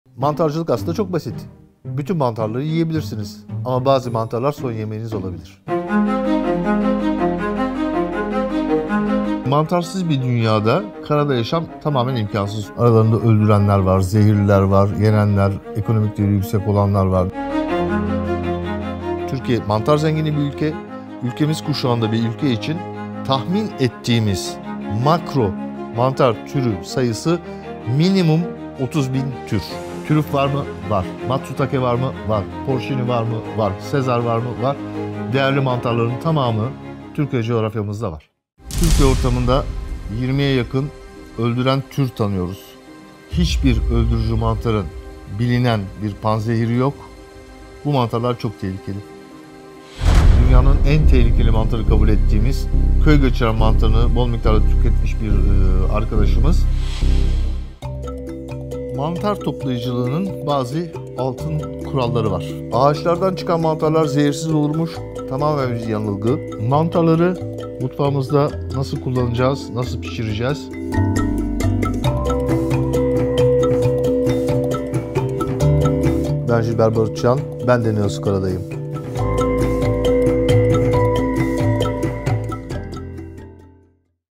Jilber Barutçiyan'dan Lezzetli, Ölümcül, Şaşırtıcı: Mantarların Dünyası
Dünyanın en tehlikeli ve en lezzetli mantarlarıyla tanışmaya hazır mısın? Mantarlar gerçekten birer protein deposu mu? Hangi mantar türü nerede aranmalı?

Mikolog Jilber Barutçiyan, Lezzetli, Ölümcül, Şaşırtıcı: Mantarların Dünyası eğitiminde tüm bu soruların cevaplarını, mantar tüketiminin püf noktalarını, doğru bilinen yanlışları ve mantar toplayıcılığının altın kurallarını paylaşıyor.

Lezzetli, Ölümcül, Şaşırtıcı: Mantarların Dünyası eğitimini hemen şimdi izle; mantarların en değerli ve en tehlikeli türlerine, doğasına, saklama ve pişirme yöntemlerine dair tüm bilmen gerekenleri keşfet!